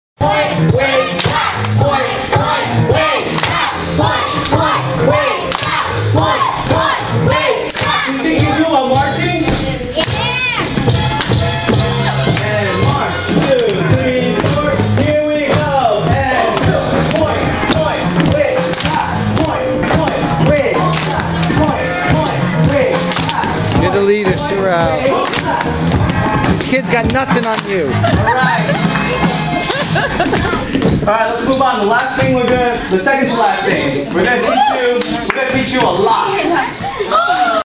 graffiti reset ministries-1/1
Graffiti Community Ministries in NYC had a great class with "Reset Ministries".